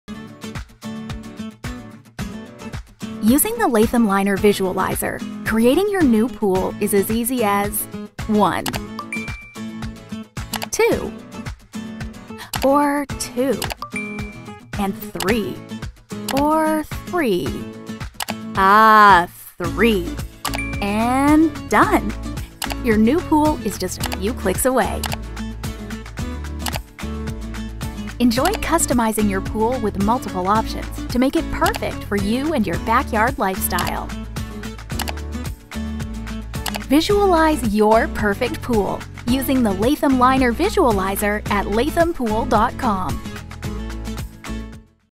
Latham Liner Visualizer Tool
Have you been daydreaming about what your fantasy pool might look like sitting in your backyard? With our new vinyl-liner visualizer, you don’t have to wonder anymore. Simply select your preferred settings and start experimenting!